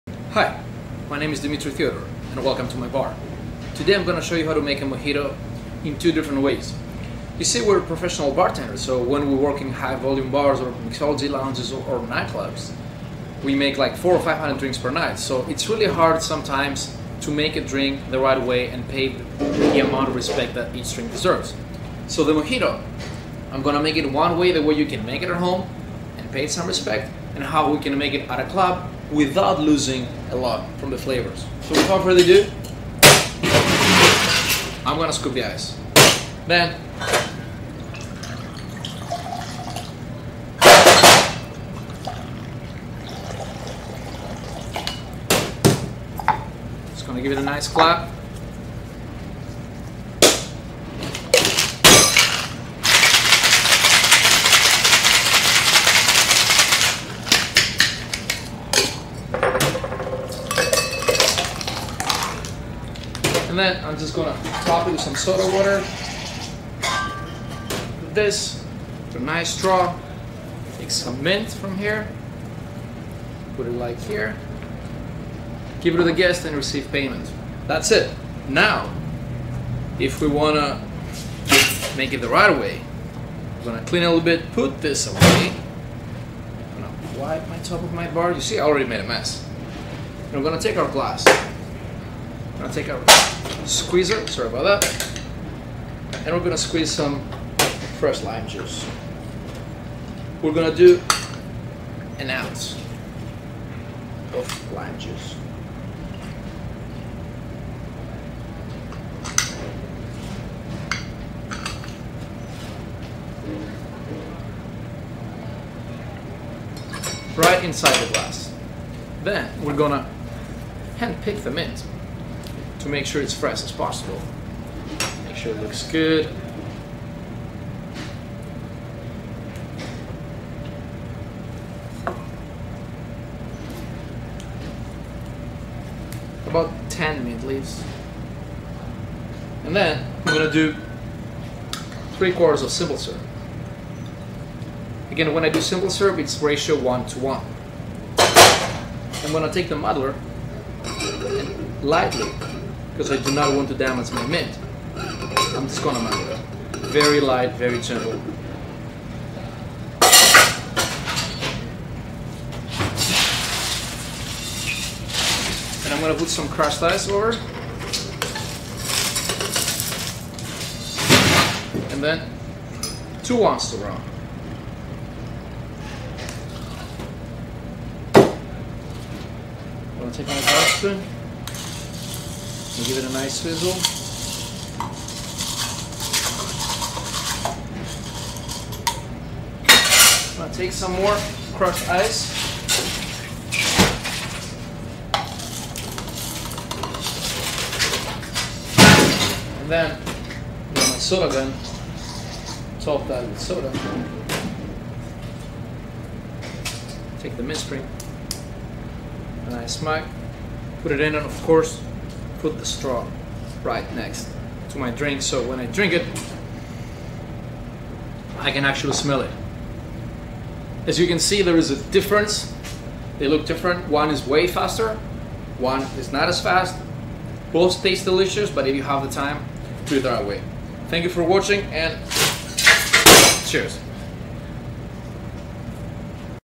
How to make a mojito. Two different ways. Dimitri Theodor - Welcome To My Bar
mojito 2 different ways
Note: in cuba the original recipe calls for yerba buena or spearmint

fast way
2 oz white rum
1 oz lime juice
1 oz simple syrup
8-15 min leaves
shake and dump. top with soda water. garnish with mint spring

proper way

2 oz white rum
1 oz lime juice
3/4 simple syrup
8-15 mint leaves muddled
crushed ice
mint spring for garnish